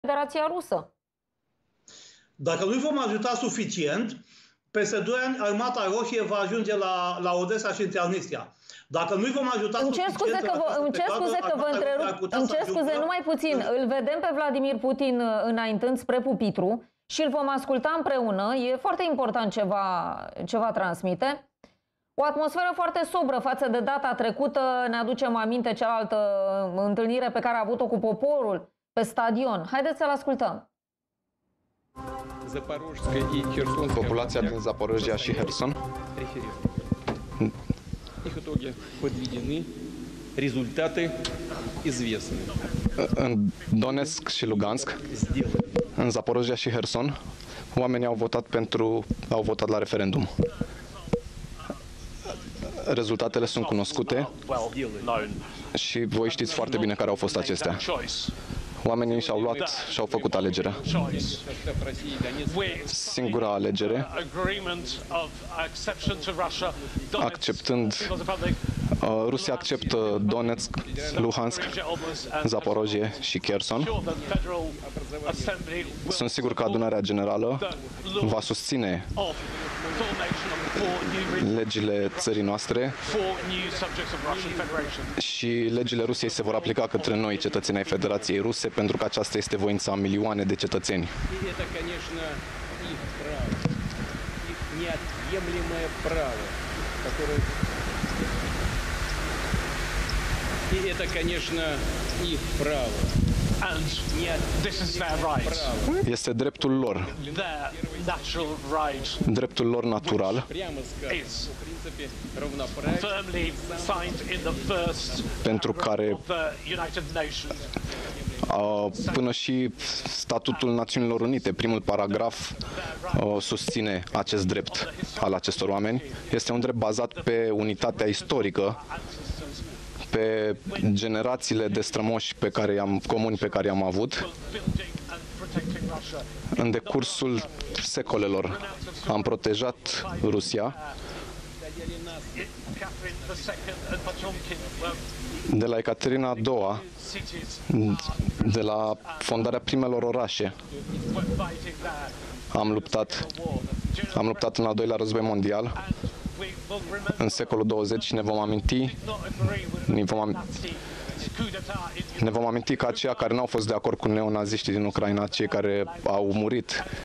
Vladimir Putin anunță anexarea regiunilor ucrainene:Rusia acceptă Donețk,Lugansk,Herson și Zaporojie
„Este voința a milioane de cetățeni. O să ne protejăm oamenii cu toate mijloacele. Vestul vrea să ne distrugă”